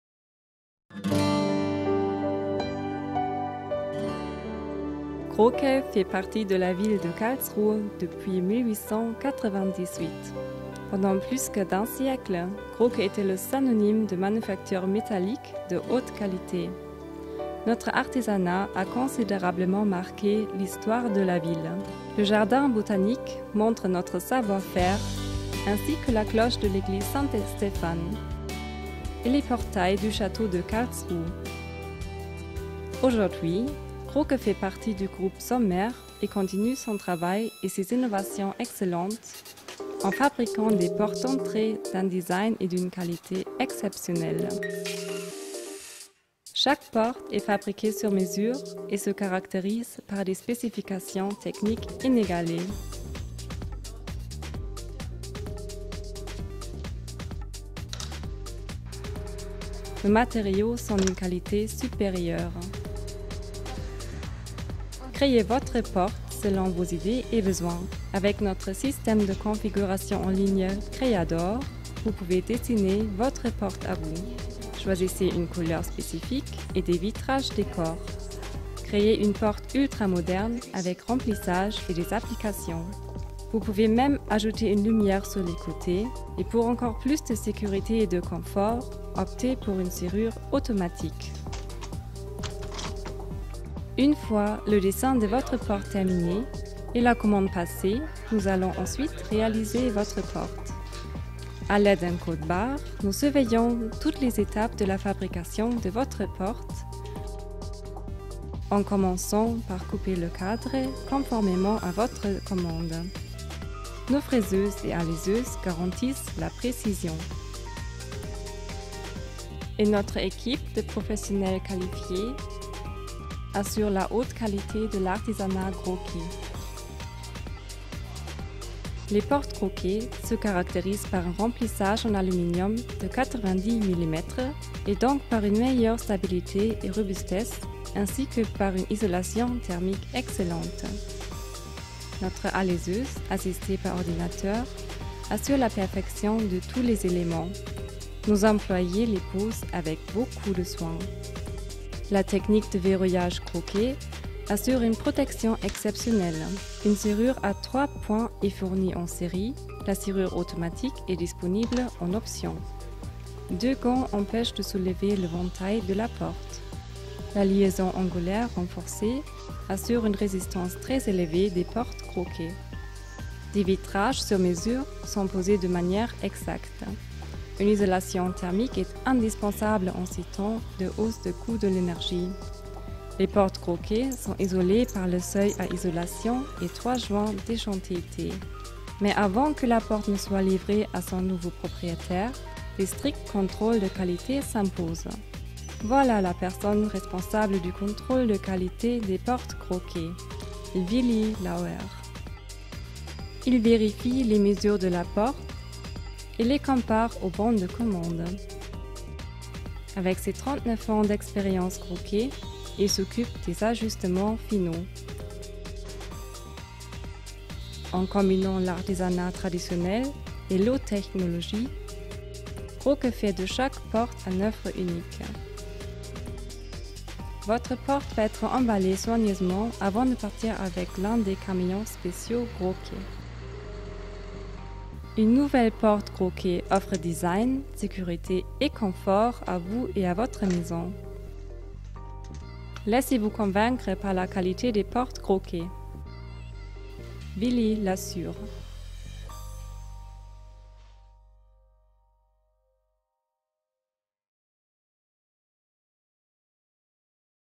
La Groke Türen GmbH se présente | Made in Germany | French
La société Groke Türen GmbH, implantée à Karlsruhe, connaît depuis 120 ans le succès dans la construction métallique. L’entreprise s’est fait un nom avec ses solutions innovantes dans le domaine des composants en aluminium.

Les produits Groke font partie du cercle restreint des produits de pointe sur le marché allemand de la construction métallique et sont fabriqués selon des normes internes très strictes. Leur design et leur sécurité font référence, tout comme leur bilan énergétique qui repose sur des systèmes de profilés en aluminium à isolation thermique spécialement conçus à cet effet. L'ouverture des portes d’entrée sans clé par empreinte digitale constitue une nouvelle étape.

À la sortie d’usine, chaque produit Groke est livré dans un emballage d’excellente qualité. Pour que les conditions et les délais de livraison convenus puissent être respectés, c’est le parc automobile de Groke qui se charge du transport.